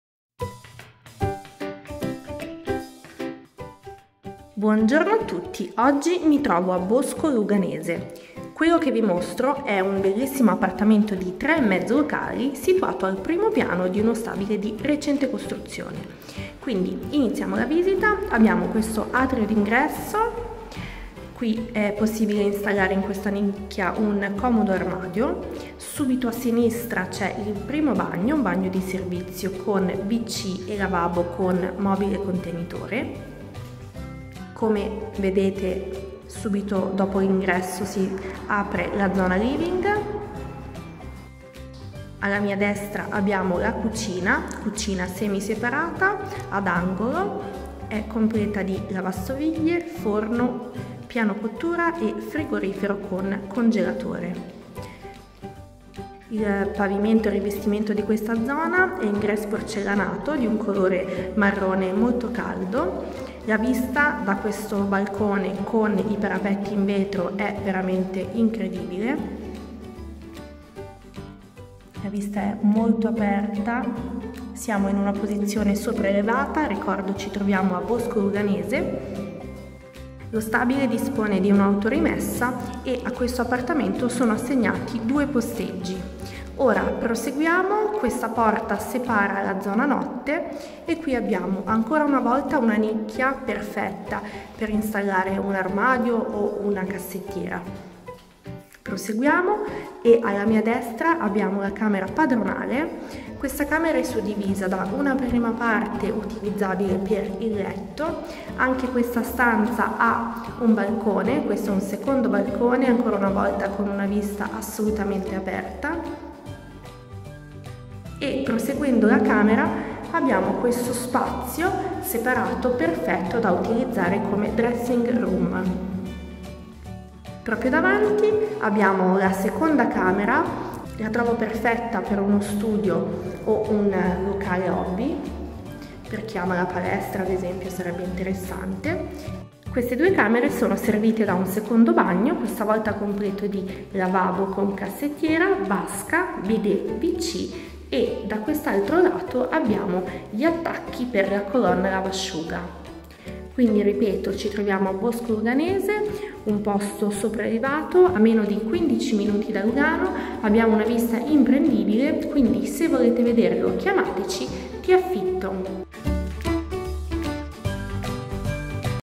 3.5 LOCALI PER COPPIE A BOSCO LUGANESE
Siamo a Bosco Luganese,
in una incredibile altura che sovrasta Lugano e regala panorami mozzafiato.

Ci troviamo in uno stabile recentemente edificato di 3 piani totali, servito da ascensore.

L'appartamento si trova AL SECONDO PIANO ed ha una superficie totale di circa 80 mq.

E’ perfetto PER UNA COPPIA che si trova alla ricerca di un sereno e moderno nido d’amore.

L’atrio precede la zona living che affaccia su grande terrazzo che mostra in tutto il suo splendore il panorama.

Immagina le serate di luna piena mentre con il tuo partner amerai sorseggiare un bicchiere di vino mentre le luci della città in lontananza si spengono pian piano.

La cucina si trova a vista rispetto al soggiorno e si sviluppa ad angolo, forno ad aria calda a mezza altezza, ampio congelatore con frigorifero, e piano cottura ad induzione con cappa.

Alla destra della cucina troviamo un BAGNO DI SERVIZIO con wc e lavabo con cassettiera.

Ora percorriamo il corridoio che serve LA ZONA NOTTE, qui è presente una nicchia che potrai utilizzare come libreria o da arredare con cassettiere e armadi.

Sulla destra c’è una PRIMA CAMERA SINGOLA ideale da utilizzare come studio o come locale hobby.

LA CAMERA PRINCIPALE ha una superficie è suddivisa in due spazi, nel primo istallerai il tuo comodo letto mentre nella seconda zona potrai installare una grande dressing room.

IL BAGNO si presenta in stile moderno è completo di vasca con poggiatesta, wc, bidet e lavabo con mobile contenitore.

In questo locale è presente l’attacco per la COLONNA LAVASCIUGA che ti permetterà di lavare direttamente in casa senza sottostare a stressanti turni condominiali.

I pavimenti della casa sono IN GRESS PORCELLANATO dal colore grigio fumo moderno ed elegante.

Gli infissi SONO IN PVC con doppi vetri e tapparelle elettriche.

IL RISCALDAMENTO è servito da serpentine.

L’affitto per questo romantico appartamento è di 1'600 più 200 di acconto spese.

Dispone di DUE POSTI IN AUTORIMESSA al costo di 200 franchi complessivi.

Le mensilità per la GARANZIA sono tre e si accettano cauzioni tramite swiss-caution.

Gli ANIMALI sono ammessi previa valutazione.

Se ti è piaciuto questo immobile e non vuoi fartelo scappare, contattaci allo oppure .